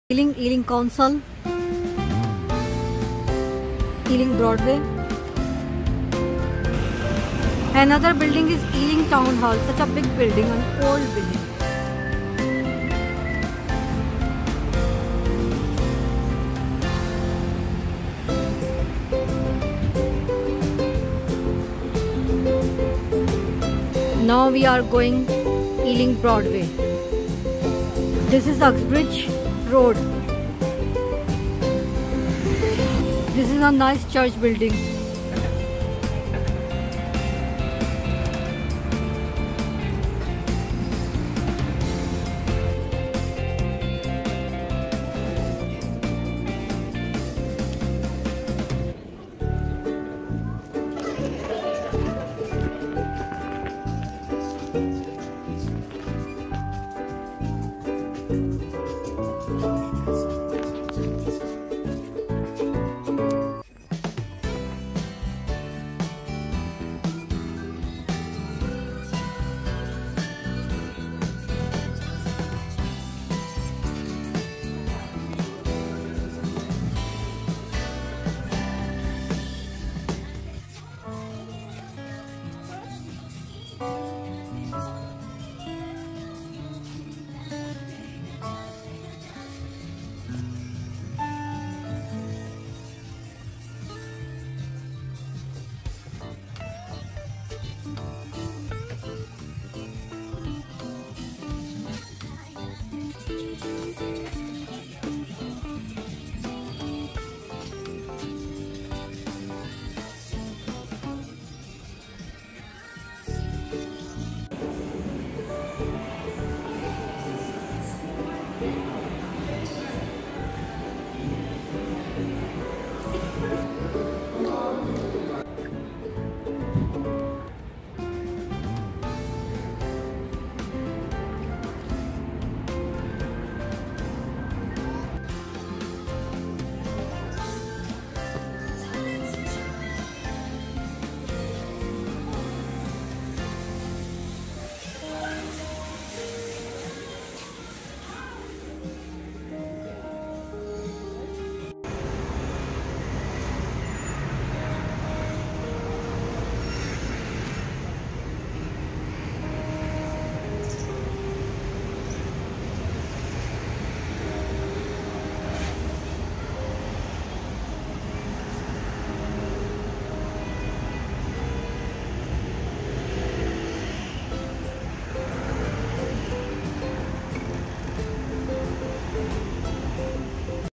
The Ealing Broadway London
Ealing council  Ealing

London Borough council is the local authority for the London Borough of Ealing in the greater London England. It is a London Borough council, one of 32 in the United Kingdom Capital of London.

Ealing town hall

Ealing town hall built in 1887 to celebrate the growth of  local democracy in the  borough, the spectacular grad all listed Victorian  building was expanded in 1930 to create the town hall building of today 120 years later  it is still providing the same public function that it was built for whether  you are planing a small at large event. Ealing town hall offers eight diverse rooms, catering for up to 450 guests , making it the ideal location for your event.

 Ealing is the best know for its film studios, which are the oldest in the world and are know especially for the Ealing comedies including kind Hearts and coronets, passport to pimlico the Lady killers and the Lavender hill Mob.

 How to far is Ealing from center London , The distance between Ealing and Hackney Central ( ward) is 11 miles, the road distances is 12.4 miles.

The Ealing Broadway Centre, currently branded as simply Ealing Broadway is a shopping centre in Ealing, west London, located on the Broadway close by. Ealing Broadway station, the centre of 85 units both indoor and courtyard part outdoor   opened 7 March 1985.

If you like this video Please press like button,